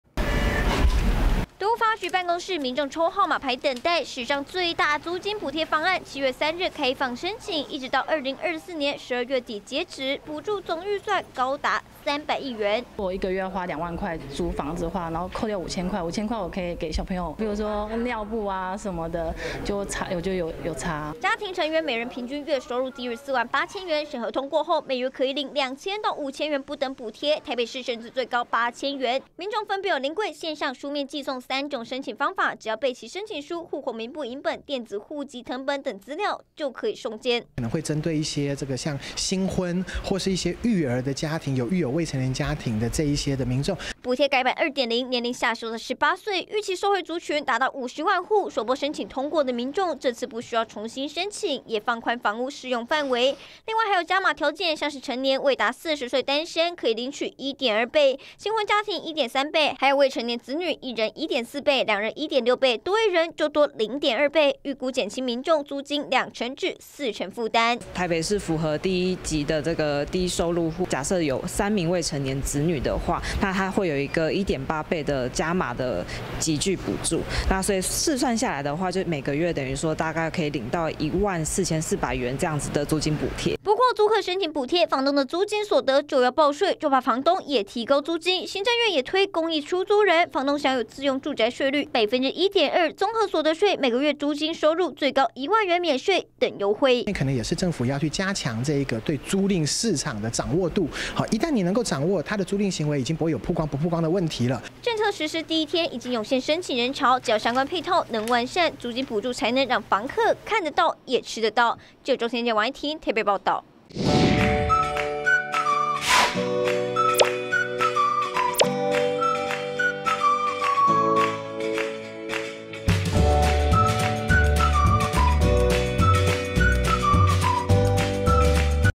【每日必看】史上最大"租金補貼"開放申請 北市1家5口省1.4萬 20230703
史上最大租金補貼方案
在今(3)日開跑
第一天就有不少民眾
跑到 都發局 辦公室臨櫃申辦
而這次租金補貼,
如果是 40歲以下單身青年,
或是新婚家庭
又或者是有撫育未成年子女等
符合條件補助金還可以加倍
像是台北市低收入戶
若養育3個未成年子女
最高每月可以補貼
超過1萬4千元